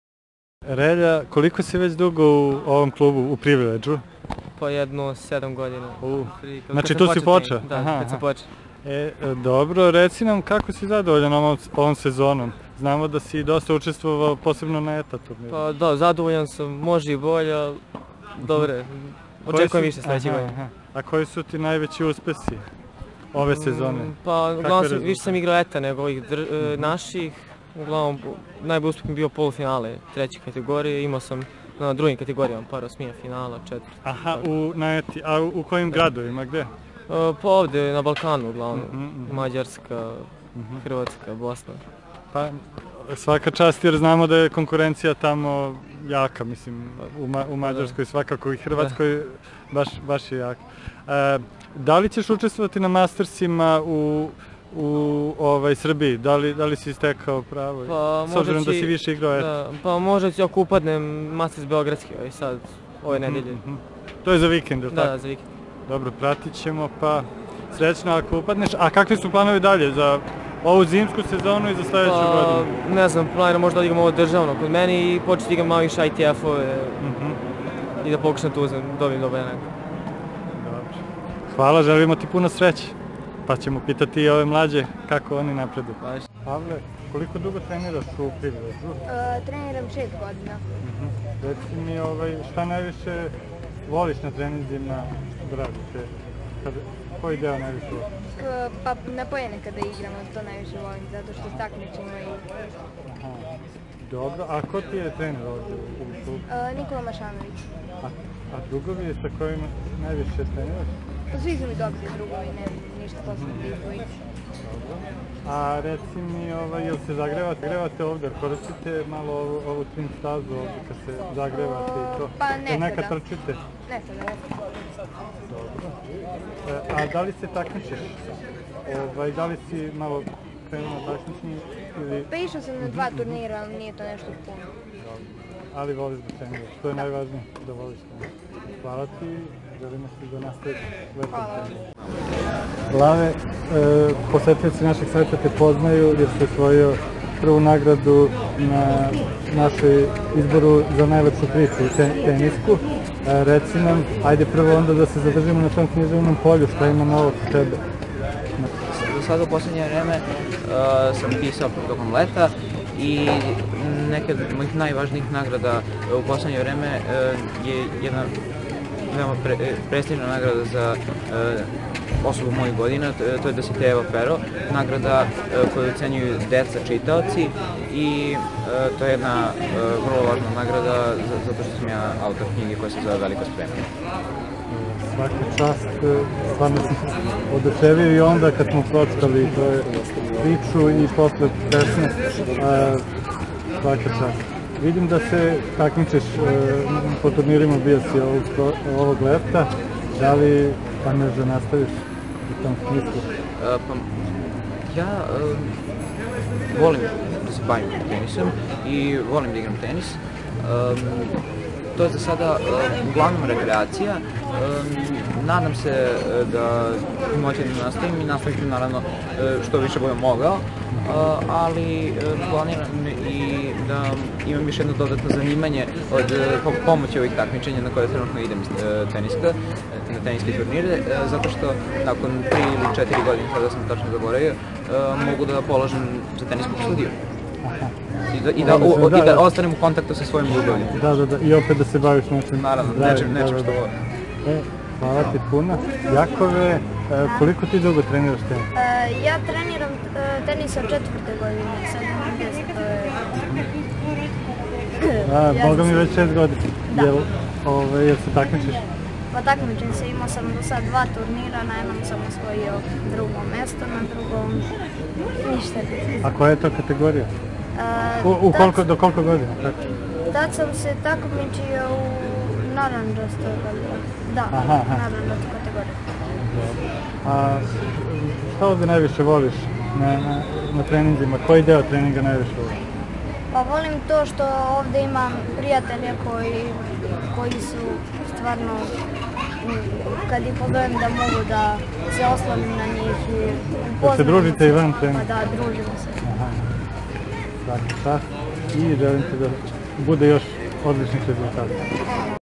Relja Eskić, Pavle Kovačević, Lav Lukić i Jakov Ljubojević, TK Privilege
Takmičare beogradskog TK Privilege Relja Eskić, Pavle Kovačević, Lav Lukić i Jakov Ljubojević pitali smo kako su zadovoljni ovom teniskom sezonom, šta najviše vole da rade na treningu, a Lav nas je obradovao novom nagradom za svoje književno stvaralaštvo - po izboru dece dobio je prestižnu nagradu „Dositejevo pero“ za svoju knjigu „Veliko spremanje“